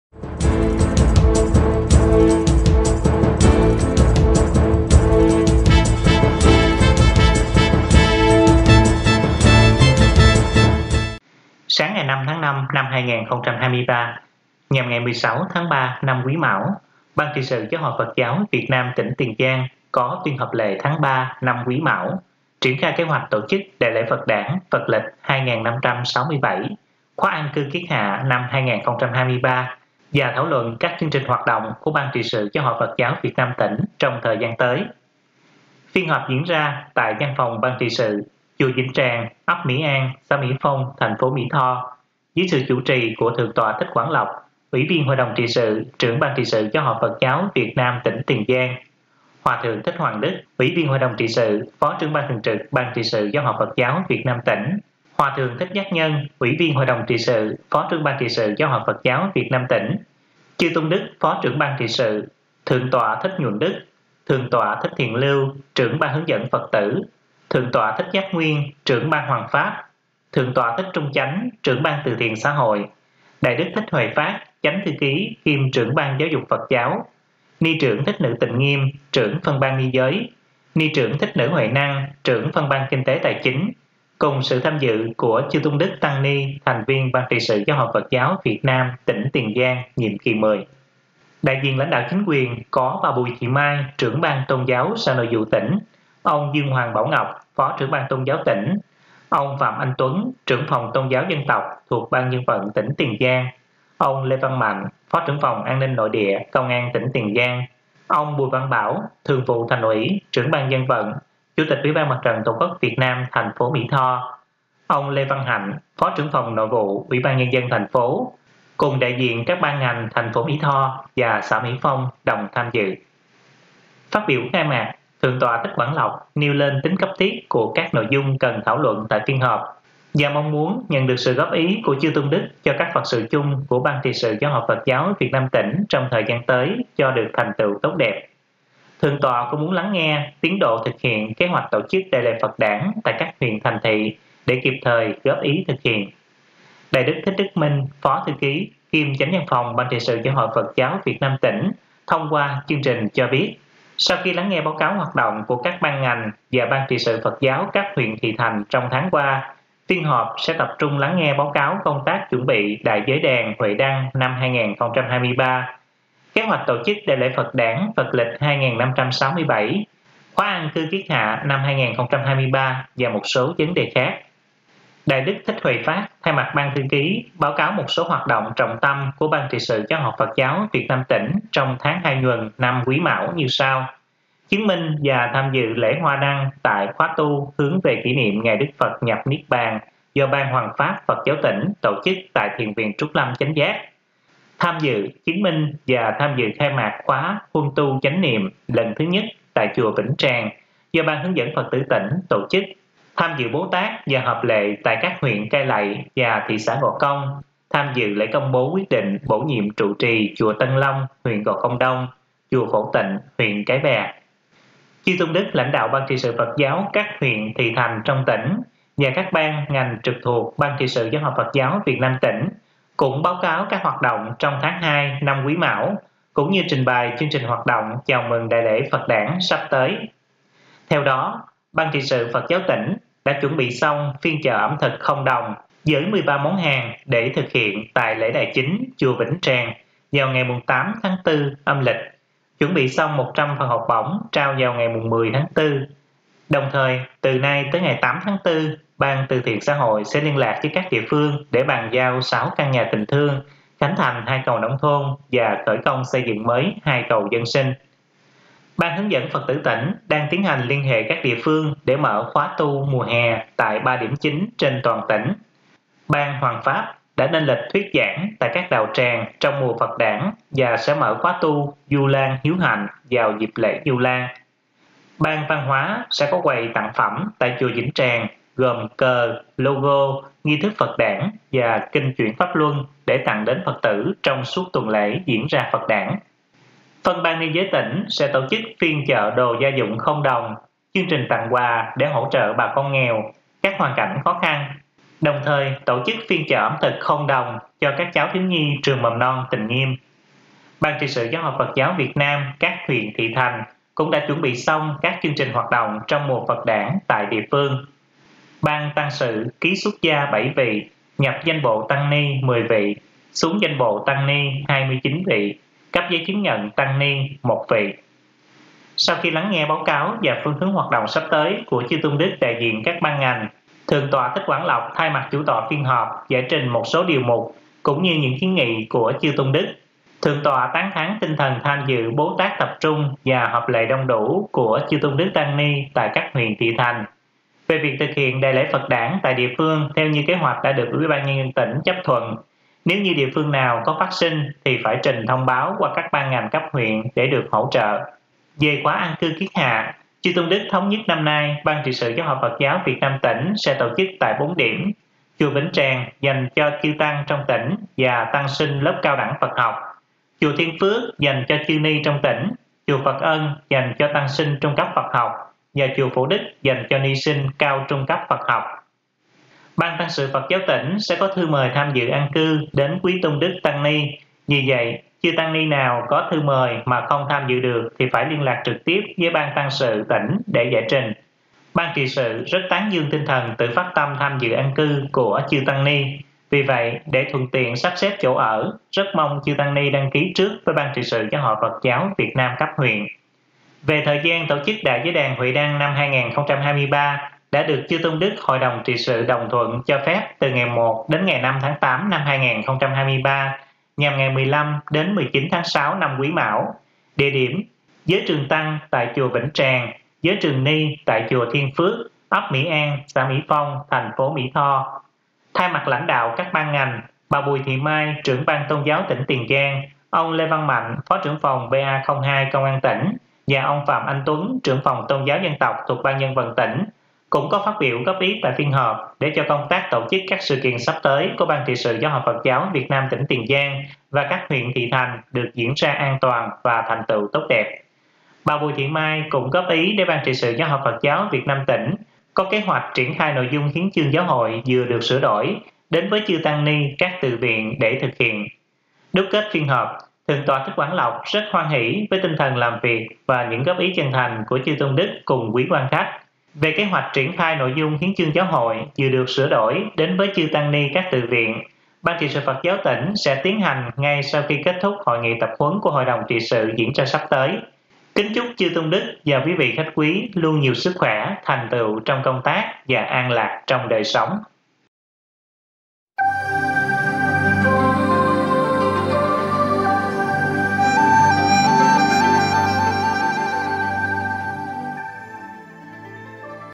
Tiền Giang: BTS Phật giáo tỉnh họp lệ triển khai kế hoạch tổ chức Đại lễ Phật đản và An cư năm 2023
Sáng ngày 5/5/2023 (16/3 năm Quý Mão), Ban Trị sự GHPGVN tỉnh Tiền Giang có phiên họp lệ tháng 3 năm Quý Mão – triển khai kế hoạch tổ chức Đại lễ Phật đản Phật lịch 2567, khóa An cư kiết hạ năm 2023 và thảo luận các chương trình hoạt động của BTS GHPGVN tỉnh trong thời gian tới.